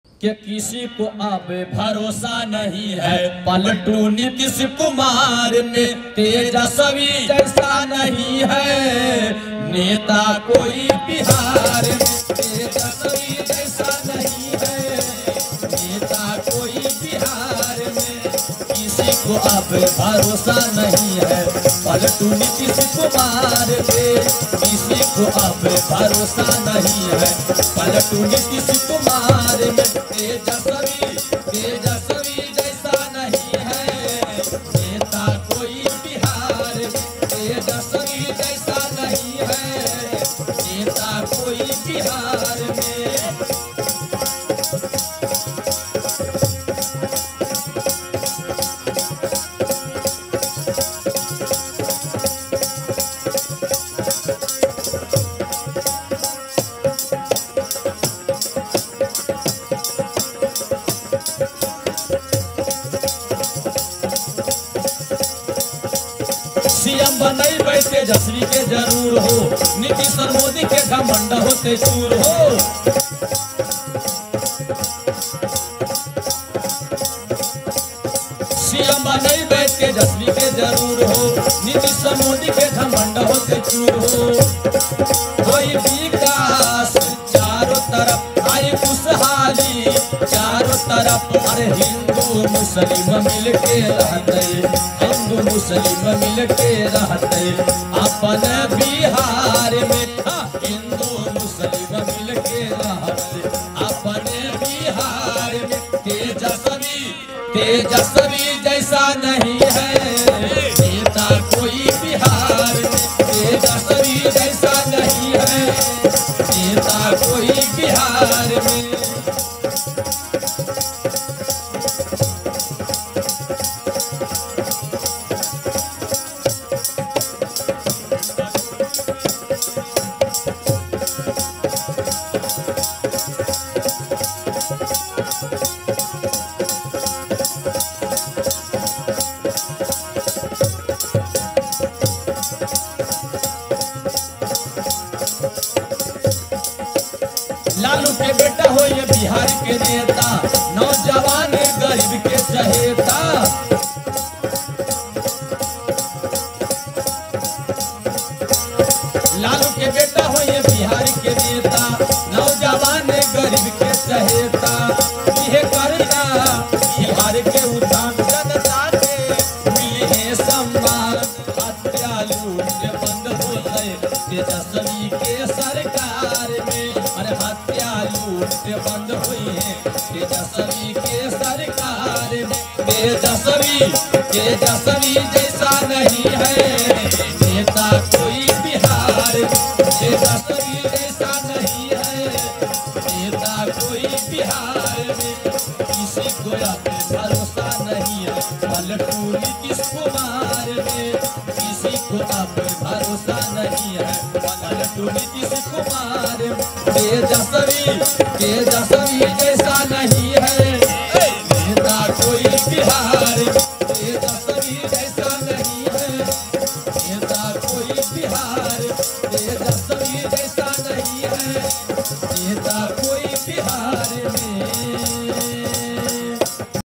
नीतीश कुमार के पलटने पर दो भाइयों ने जबरदस्त धोया ? राजद के लोग खूब कर रहे शेयर
After Nitish Kumar's election, two brothers sang in such a way that it went viral? RJD people are sharing a lot